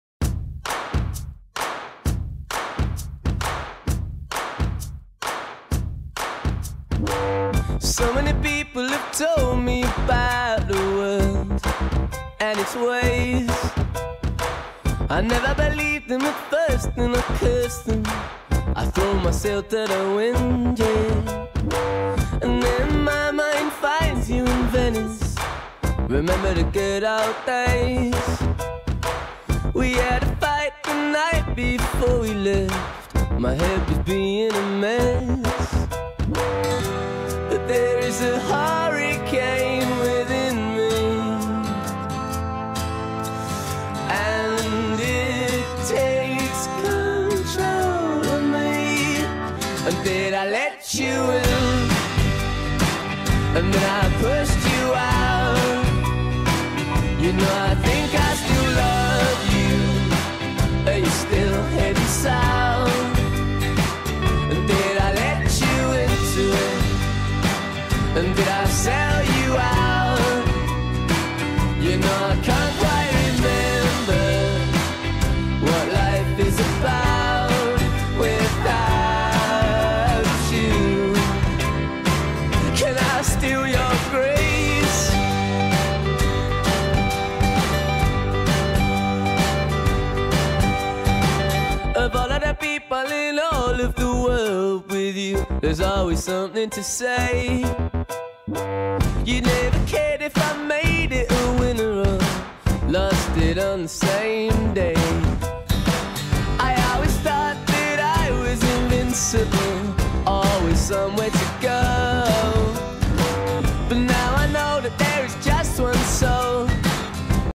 Kellar: there's a (hurricane) within me
clips from jump derby, jumper show, jump lesson, and jimmy wofford clinic!
he was so awesome!!